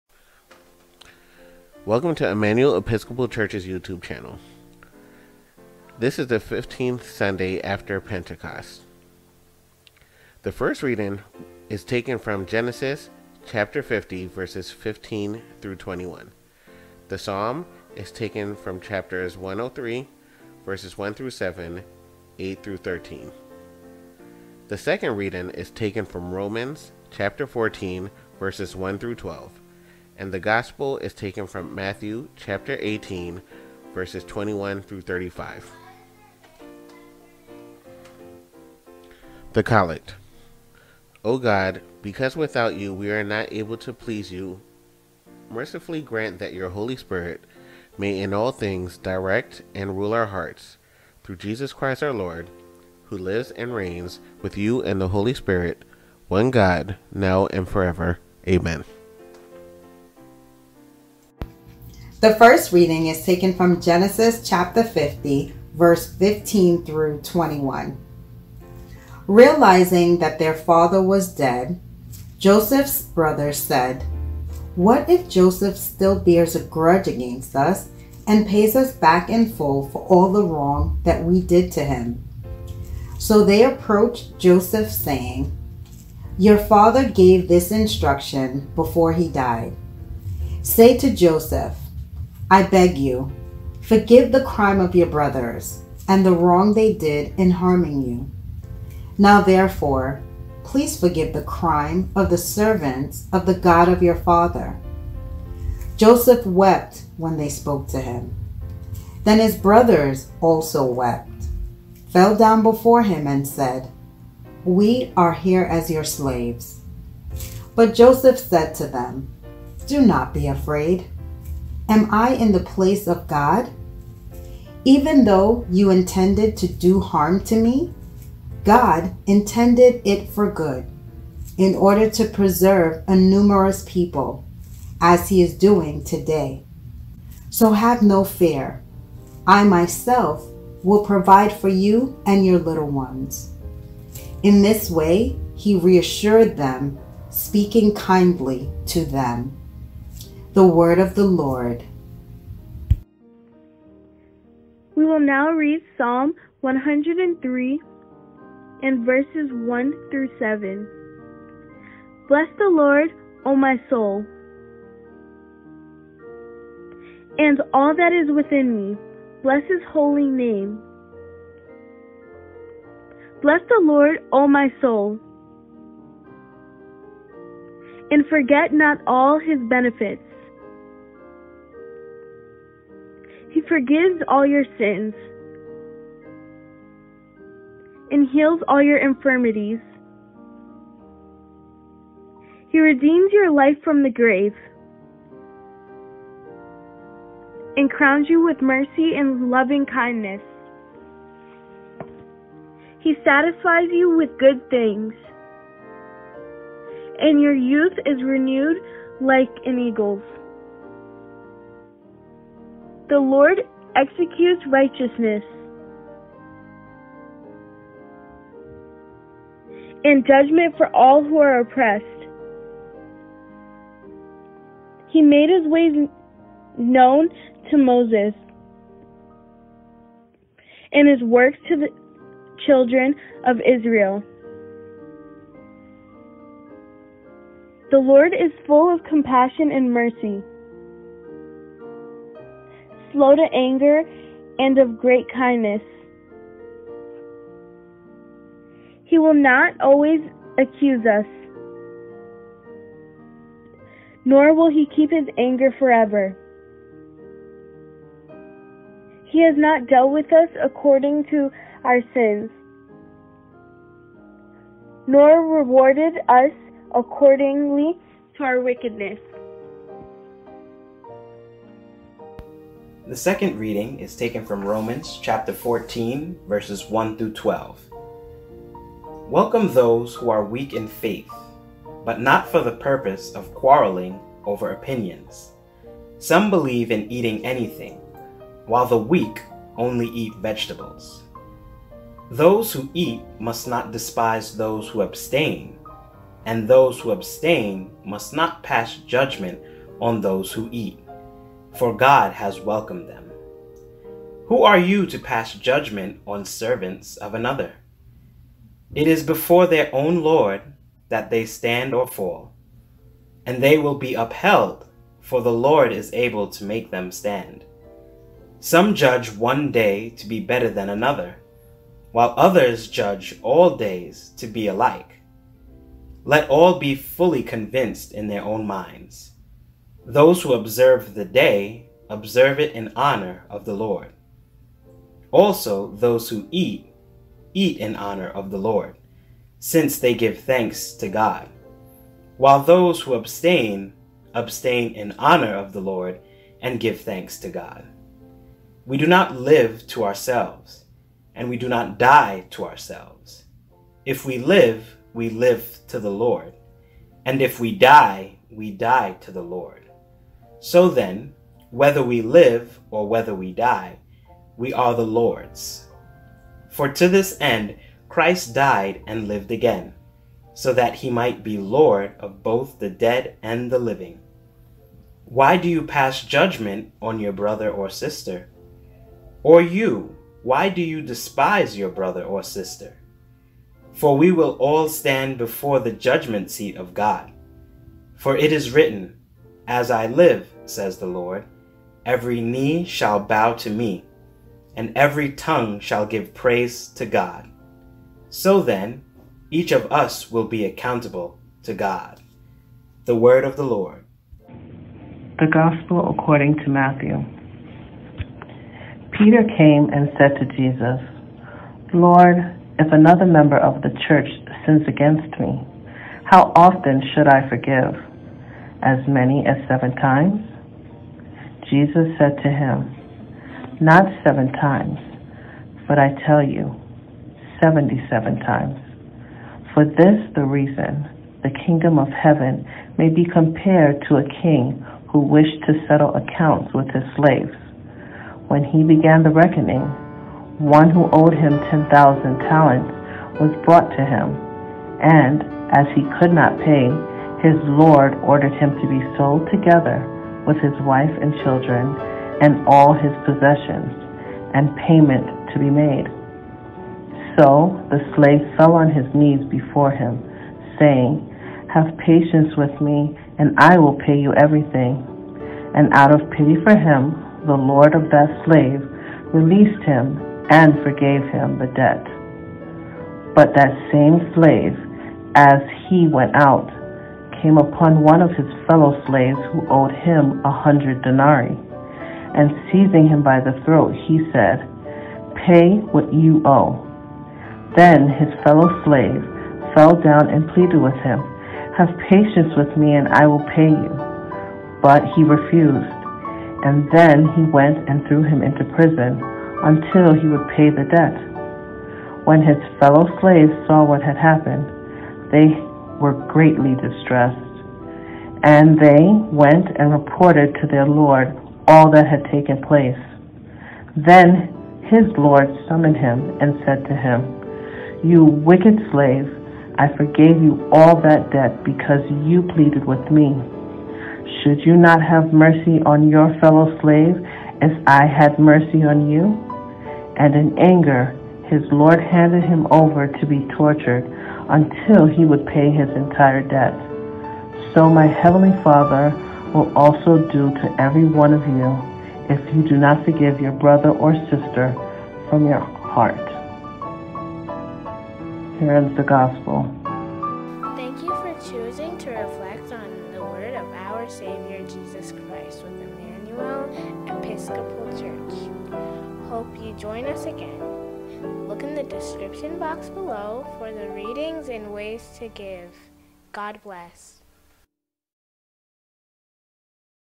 15th Sunday After Pentecost
15th Sunday After Pentecost

First reading Genesis 50:15-21
Psalm 103: 1-7, 8-13
Second Reading Romans 14:1-12
Gospel Matthew 18:21-35

Contributions and offering may be made

                                or
          Mail checks payable to :
                      Emmanuel Episcopal Church
                      2635 E 23rd St

Music: Credit to Elaine Johnson for the hymns played in this video.  Copyright by Elaine Johnson at HymnServe.com